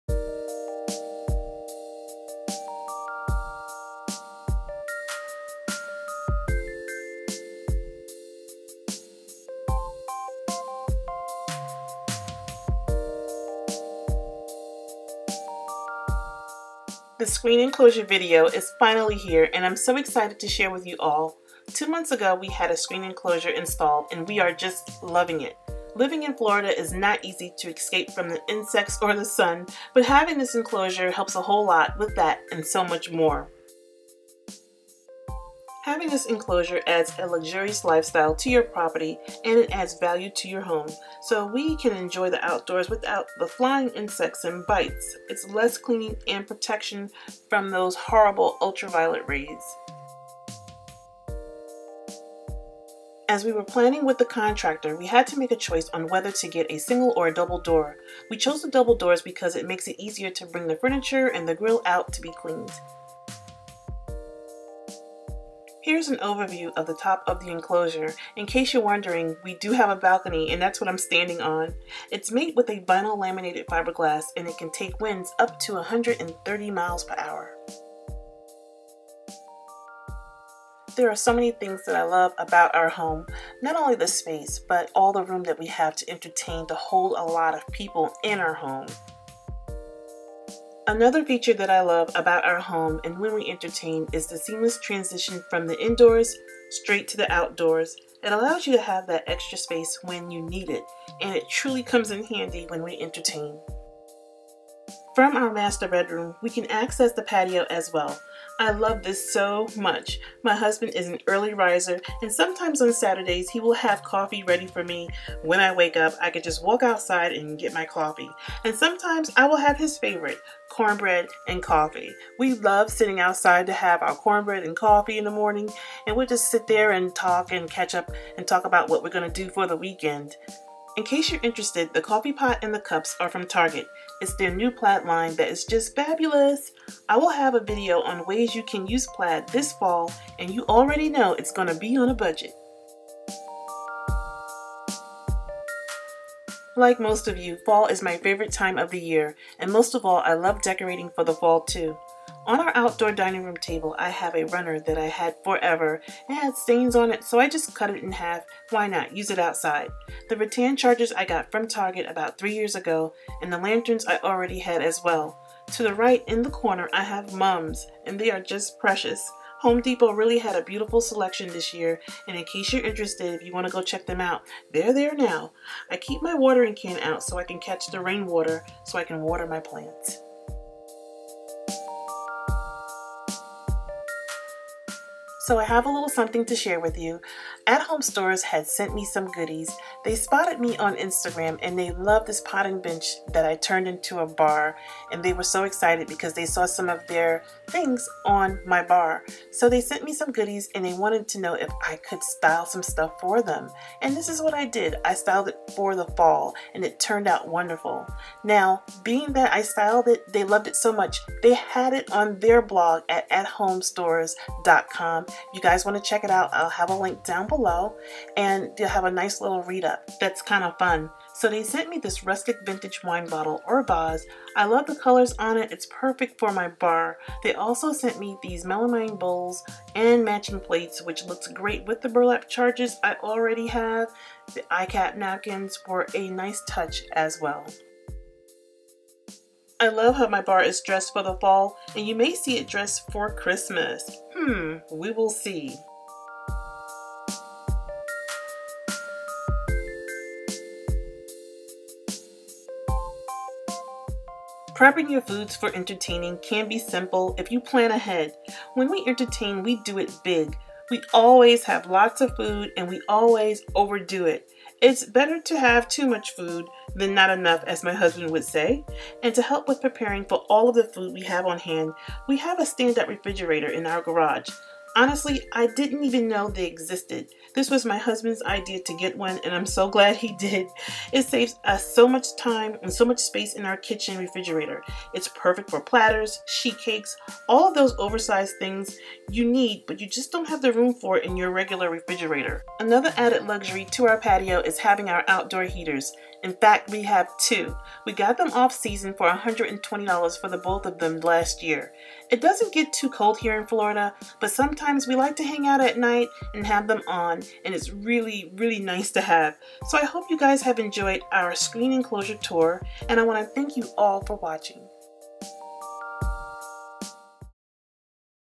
Outdoor Screen Enclosure and Fall Decor Tour
Thumbs Up! Outdoor Screen Enclosure Tour is finally up! Come see all of the details. Please enjoy!